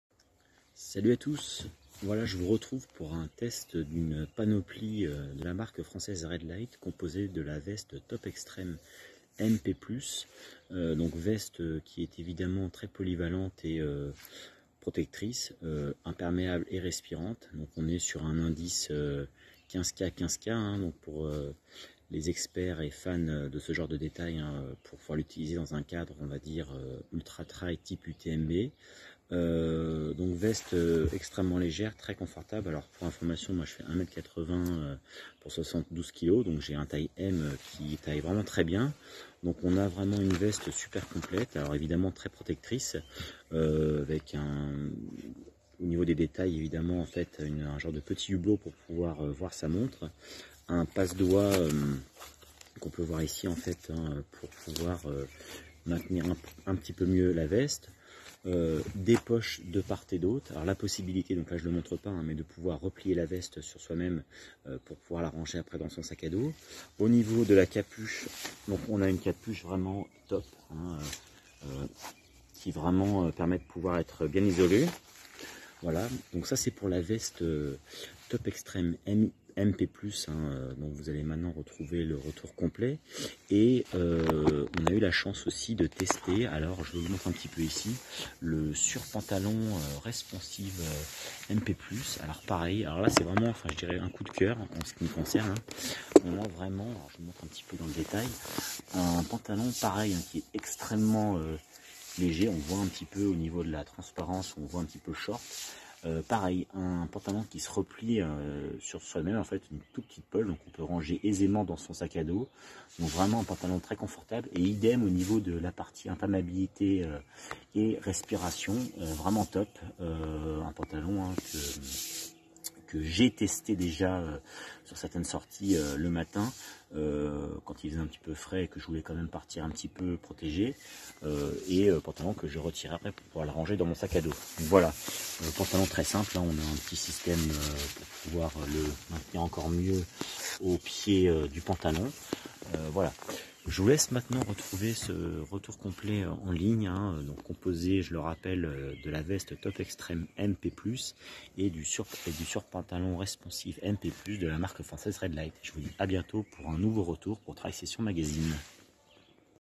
RAIDLIGHT Veste Top Extreme MP+ et surpantalon Responsiv MP+ [ Test & Avis ] ☞ par Sébastien
©TRAIL SESSION MAGAZINE 🇫🇷 French Digital Media 🇫🇷